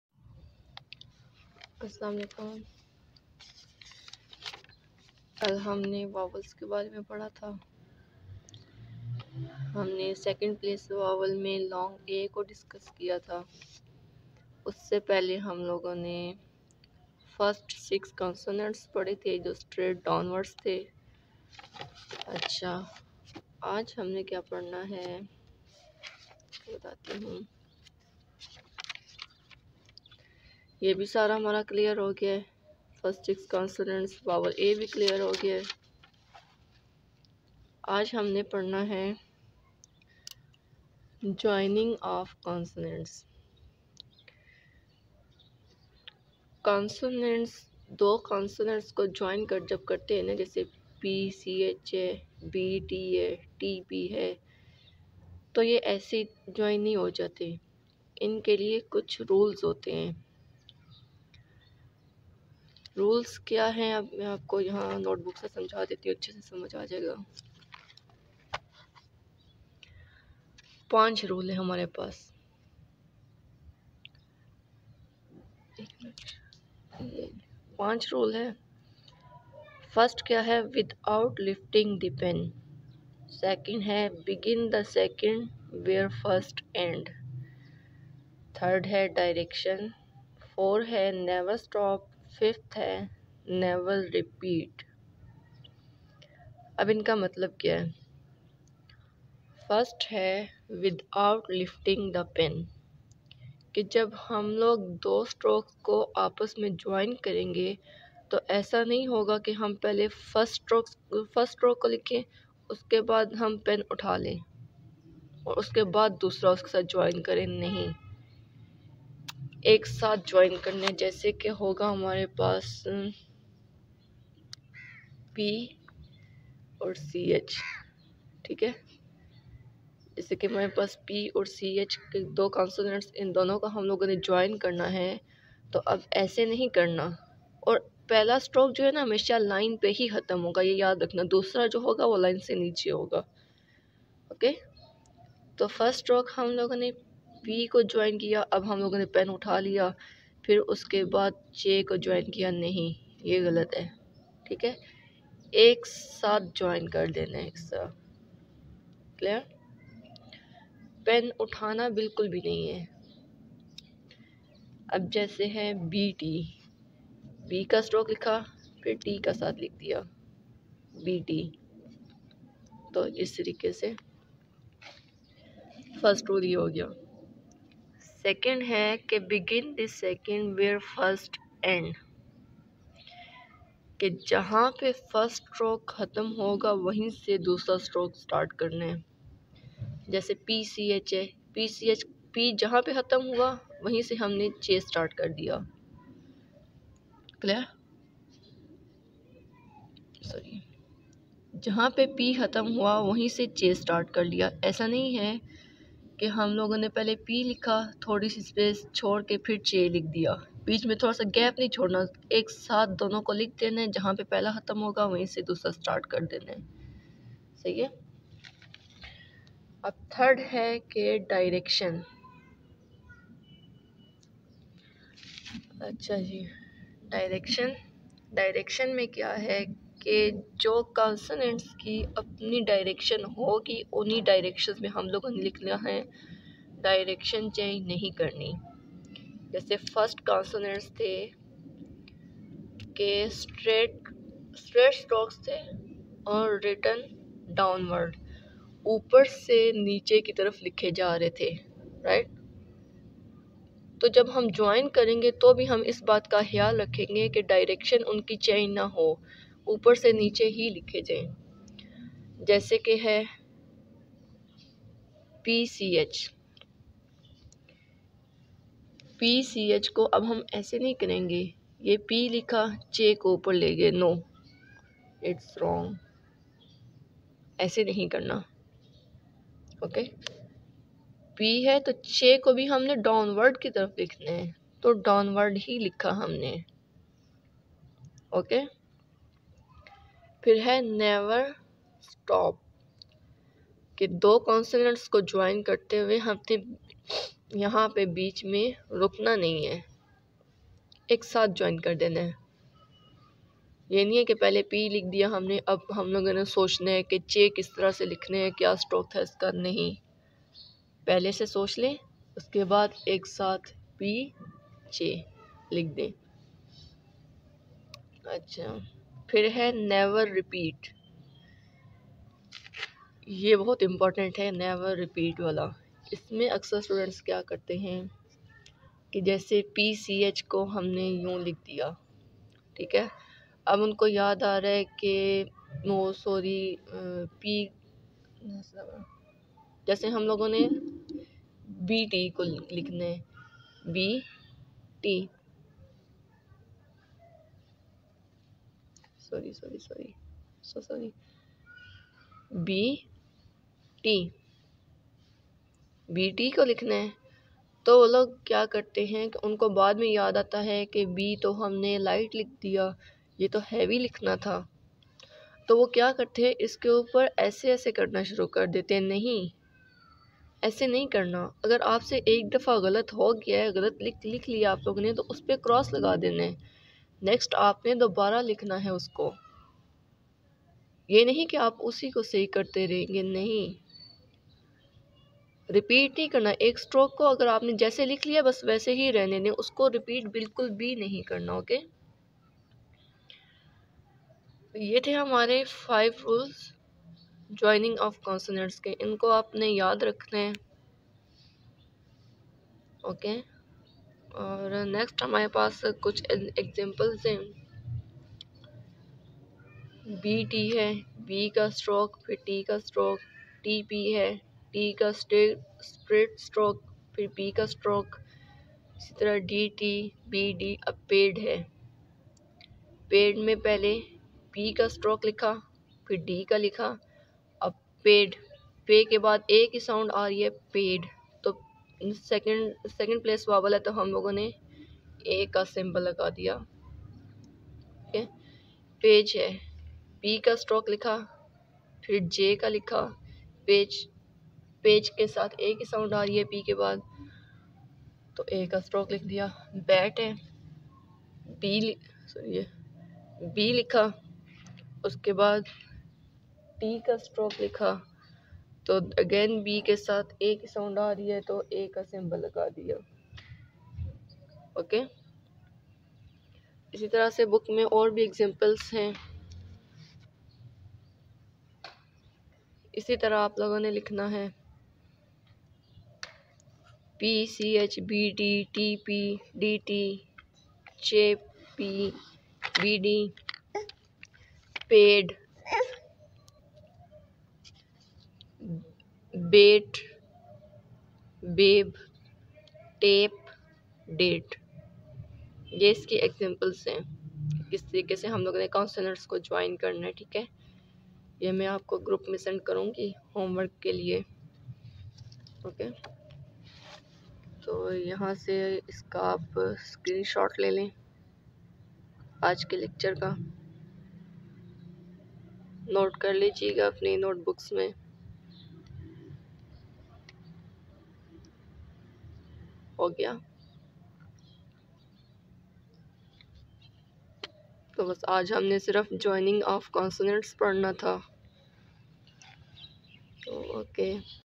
Third Lecture Of First Chapter
Is video men mene apko Joining of Consonants k Rules Sikhaye hen